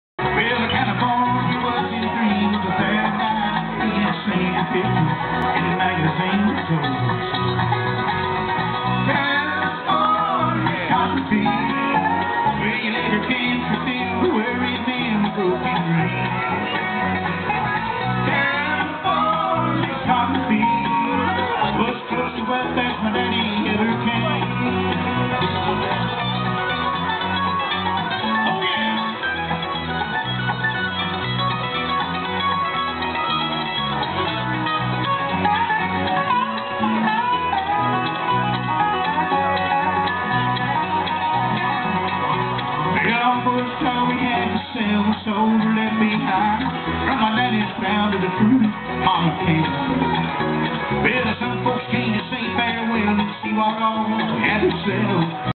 Marty Raybon and Full Circle
Footage from the Great Hahira Pick In bluegrass festival held in Hahira, GA on Nov. 7, 2009.